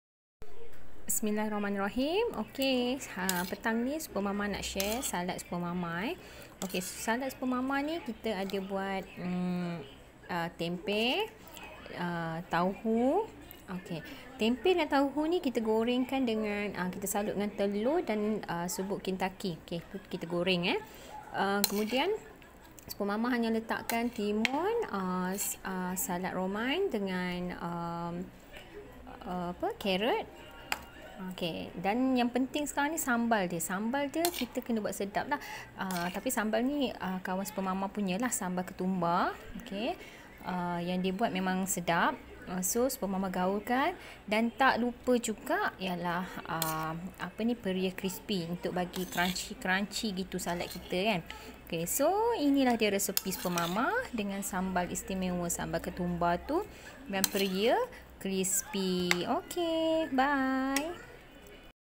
SALAD PERIA CRISPY MUDAH DAN CEPAT BERSOS KETUMBAR
Resepi mudah untuk semua nikmati..apapun pastikan sos ketumbar yang uolls semua buuat mesti sedap dan boleh di makan dengan Vietnamese Roll 🤭